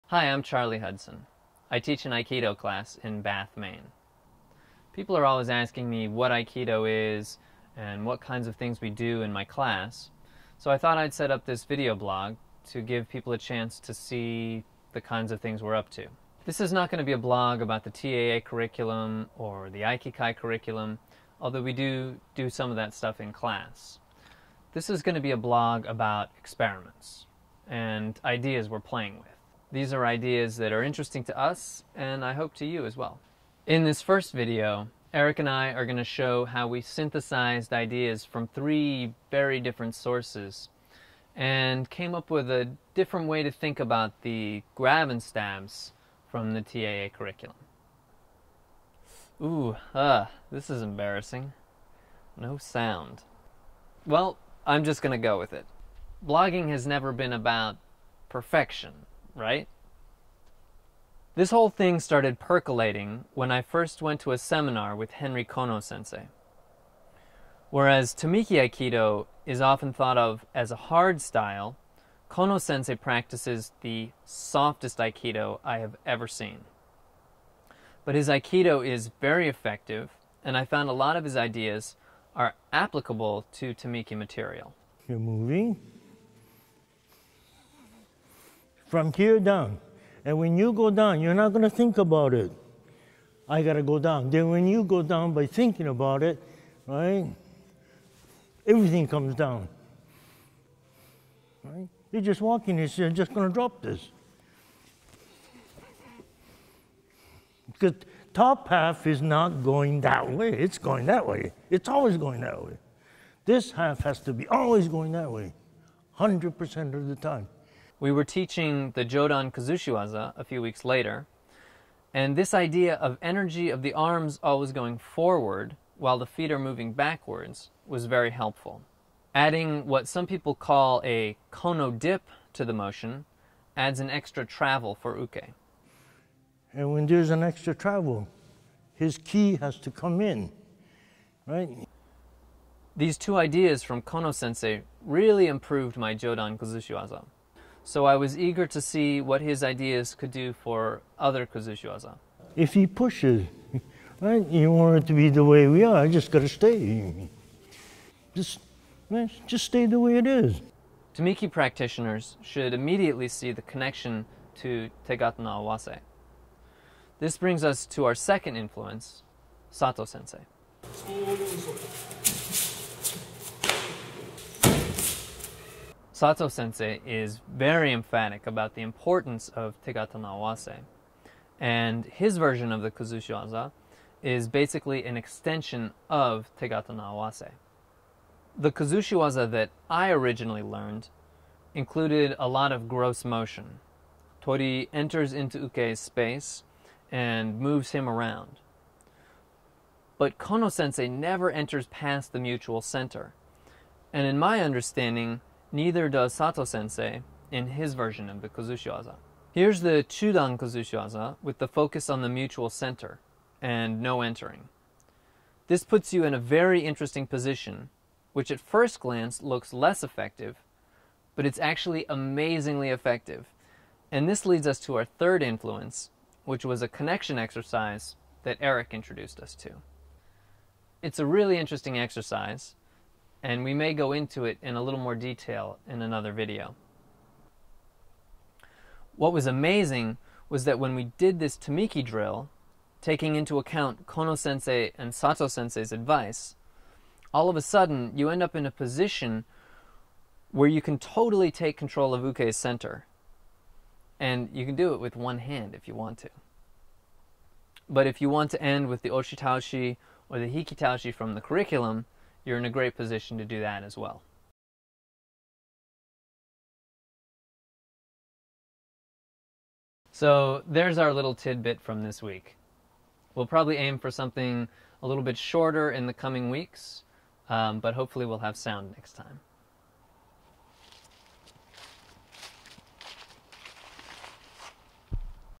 Episode 1: "Grab and Stabs" from new angles
In this first episode of the 207 Aikido podcast I talk about how Kono sensei influenced my understanding of Sato sensei's version of the jodan and chudan kuzushiwaza, and how that linked to a connection drill that Eric introduced us to. There was a major SNAFU with the sound, but I decided to just go with it.